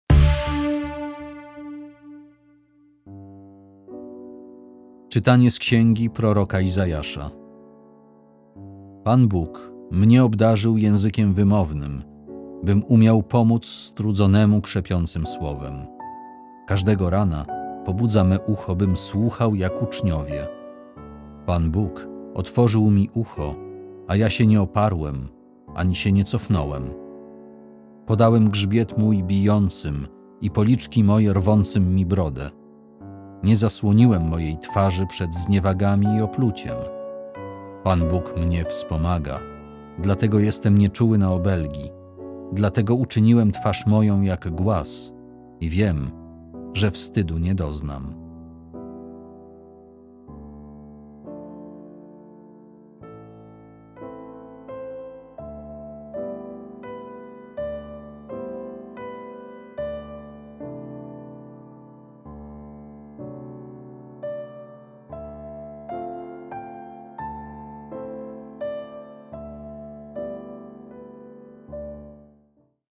PierwszeCzytanie | 5 kwietnia 2020 | Niedziela Palmowa
Pan Bóg mnie obdarzył językiem wymownym, bym umiał pomóc strudzonemu krzepiącym słowem. Każdego rana pobudza me ucho, bym słuchał jak uczniowie. Pan Bóg otworzył mi ucho, a ja się nie oparłem ani się nie cofnąłem.
Podałem grzbiet mój bijącym i policzki moje rwącym mi brodę. Nie zasłoniłem mojej twarzy przed zniewagami i opluciem.
Pan Bóg mnie wspomaga, dlatego jestem nieczuły na obelgi, dlatego uczyniłem twarz moją jak głaz i wiem, że wstydu nie doznam.